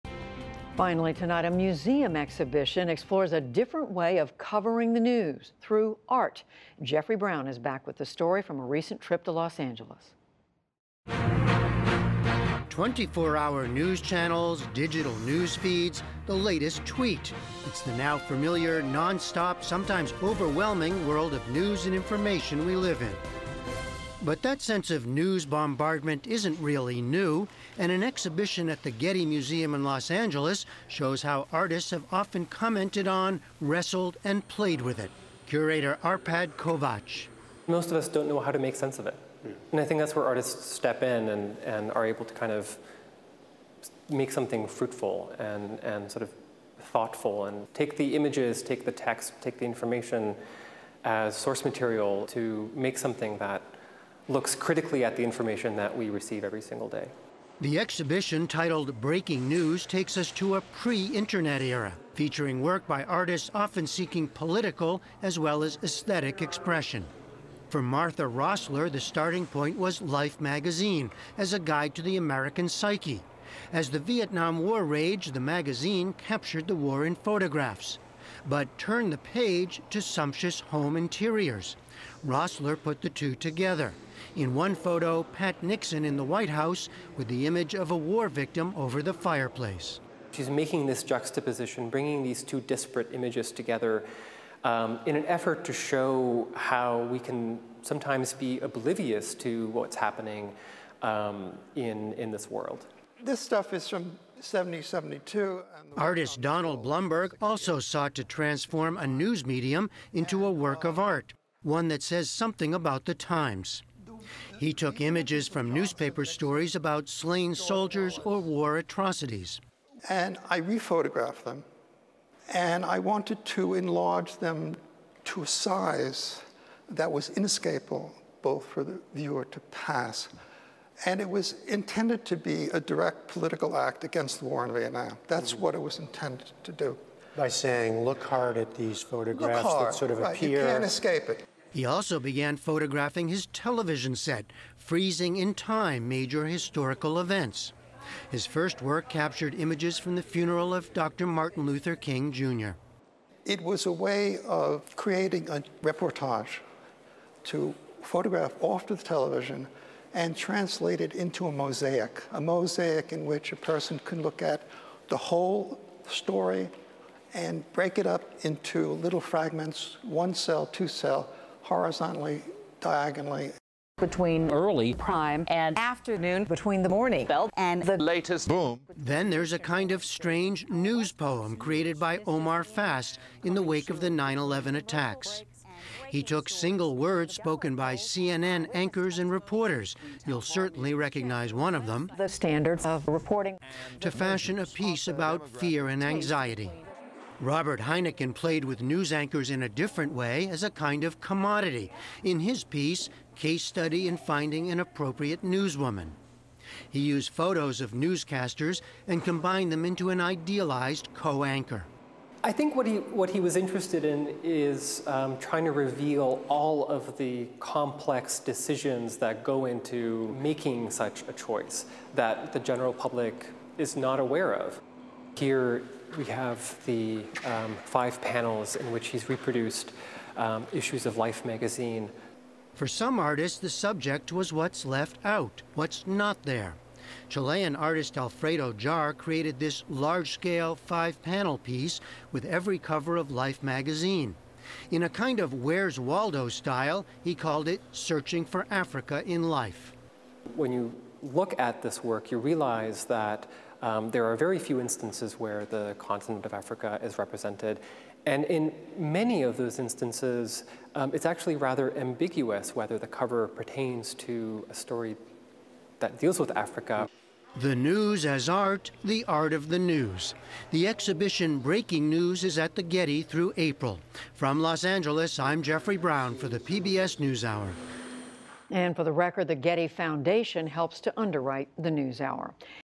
How artists have wrestled with nonstop news
An exhibit at the Getty Museum in Los Angeles takes an artistic route when it comes to covering the news. “Breaking News” takes audiences to a time back before the Internet, often featuring artists who sought political as well as aesthetic expression. Jeffrey Brown reports.